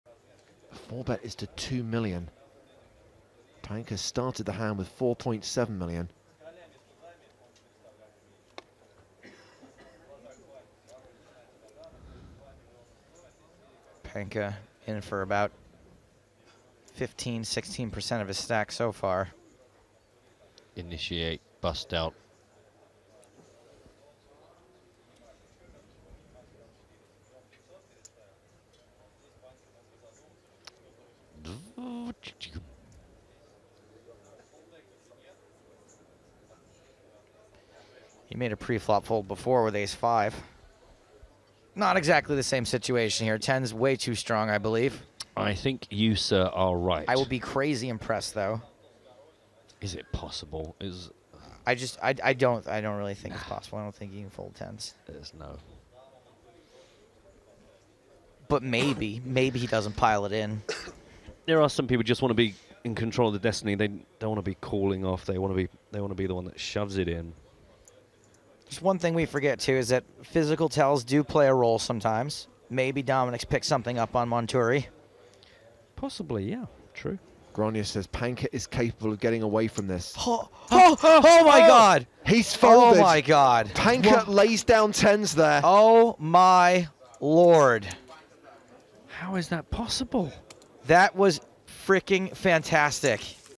Amazing Lay-down by Dominik Panka During EPT 11 Malta 2015 | PokerStars
Amazing lay-down by Dominik Panka during EPT 11 Malta 2015 Keep it cool, Keep it fun, Play Responsibly